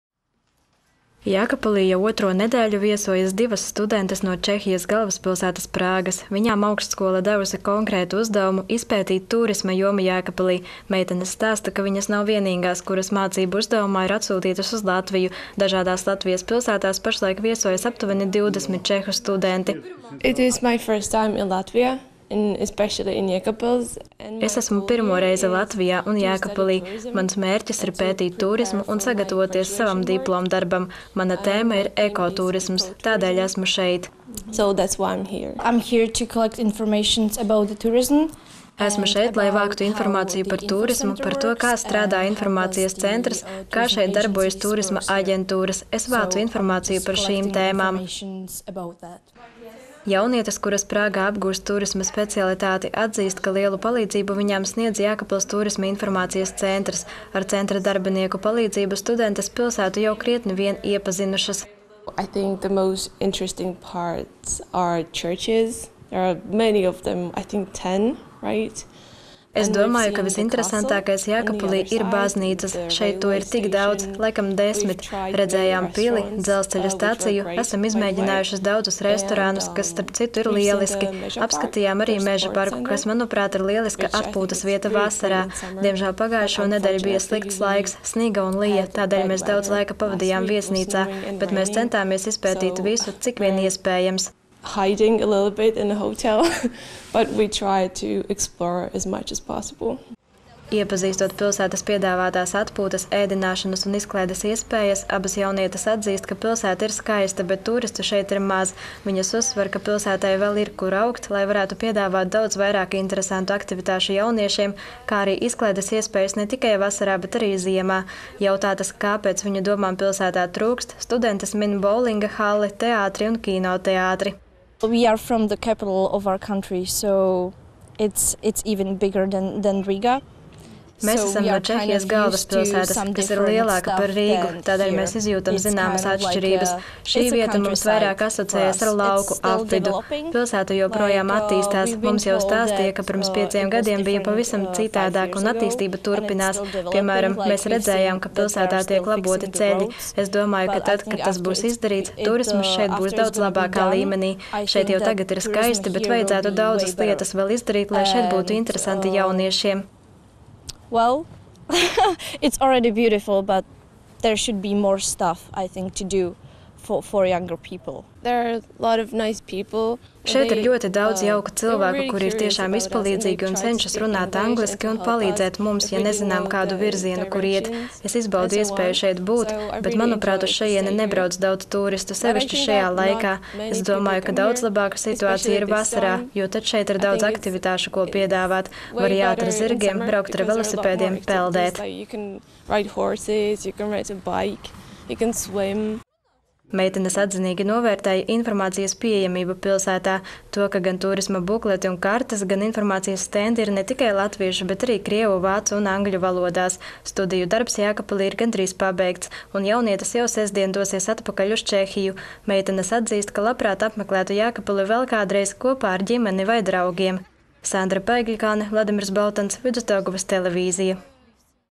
Studentes no Čehijas pēta Jēkabpils tūrismu
Jēkabpilī jau otro nedēļu viesojas divas studentes no Čehijas galvaspilsētas Prāgas. Viņām augstskola devusi konkrētu uzdevumu -- izpētīt tūrisma jomu Jēkabpilī. Meitenes stāsta, ka viņas nav vienīgās, kuras mācību uzdevumā ir atsūtītas uz Latviju -- dažādās Latvijas pilsētās pašlaik viesojas aptuveni 20 čehu studenti.

    Barbora Šťasná stāsta: "Es esmu pirmo reizi Latvijā un Jēkabpilī. Mans mērķis ir pētīt tūrismu un sagatavoties savam diplomdarbam. Mana tēma ir eko tūrisms. Tādēļ esmu šeit."

    Otra studente Bára Schutová arī pēta tūrismu, tikai mazliet citādākā aspektā: „Esmu šeit, lai vāktu informāciju par tūrismu, par to, kā strādā informācijas centrs un kā šeit darbojas tūrisma aģentūras. Es vācu informāciju par šīm tēmām."

    Jaunietes, kuras Prāgā apgūst tūrisma specialitāti, atzīst, ka lielu palīdzību viņām sniedz Jēkabpils Tūrisma informācijas centrs. Ar centra darbinieku palīdzību studentes pilsētu jau krietni vien iepazinušas.

    Barbora Šťasná saka: „Es domāju, ka visinteresantākais Jēkabpilī ir baznīcas -- šeit to ir tik daudz. Redzējām pili, dzelzceļa staciju, esam izmēģinājušas daudzus restorānus, kas ir lieliski. Apskatījām arī Mežaparku, kas, manuprāt, ir lieliska atpūtas vieta vasarā. Diemžēl pagājušonedēļ bija slikts laiks -- sniga un lija, tādēļ mēs daudz laika pavadījām viesnīcā, bet mēs centāmies izpētīt visu, cik vien iespējams."

    Iepazīstot pilsētas piedāvātās atpūtas, ēdināšanas un izklaides iespējas, abas jaunietes atzīst, ka pilsēta ir skaista, bet tūristu šeit ir maz. Viņas uzsver, ka pilsētai vēl ir, kur augt, lai varētu piedāvāt daudz vairāk interesantu aktivitāšu jauniešiem, kā arī izklaides iespējas ne tikai vasarā, bet arī ziemā. Jautātas, kā pēc viņu domām pilsētā trūkst, studentes min boulinga halli, teātri un kinoteātri.

    Bára Schutová izsaka savu viedokli: „Mēs esam no Čehijas galvaspilsētas, kas ir lielāka par Rīgu. Tādēļ mēs izjūtam zināmas atšķirības. Šī vieta mums vairāk asociējas ar lauku apvidu. Jūsu pilsēta joprojām attīstās. Mums jau stāstīja, ka pirms pieciem gadiem bija pavisam citādāk, un attīstība turpinās. Piemēram, mēs redzējām, ka pilsētā tiek laboti ceļi. Es domāju, ka tad, kad tas būs izdarīts, tūrisms šeit būs daudz labākā līmenī. Šeit jau tagad ir skaisti, bet vajadzētu daudzas lietas vēl izdarīt, lai šeit būtu interesanti jauniešiem."

    Barbora Šťasná uzsver vietējo iedzīvotāju atsaucību: "Šeit ir ļoti daudz jauku cilvēku, kuri ir tiešām izpalīdzīgi, cenšas runāt angliski un palīdzēt mums, ja nezinām kādu virzienu, kur iet. Es izbaudu iespēju būt šeit, bet, manuprāt, uz šejieni nebrauc daudz tūristu, sevišķi šajā laikā. Es domāju, ka daudz labāka situācija ir vasarā, jo tad šeit ir daudz aktivitāšu, ko piedāvāt. Var jāt ar zirgiem, braukt ar velosipēdiem, peldēt."

    Meitenes atzinīgi novērtēja informācijas pieejamību pilsētā -- to, ka gan tūrisma bukleti un kartes, gan informācijas stendi ir ne tikai latviešu, bet arī krievu, vācu un angļu valodās. Studiju darbs Jēkabpilī ir gandrīz pabeigts, un jaunietes jau sestdien dosies atpakaļ uz Čehiju. Meitenes atzīst, ka labprāt apmeklētu Jēkabpili vēl kādreiz kopā ar ģimeni vai draugiem.

    Sandra Paegļkalne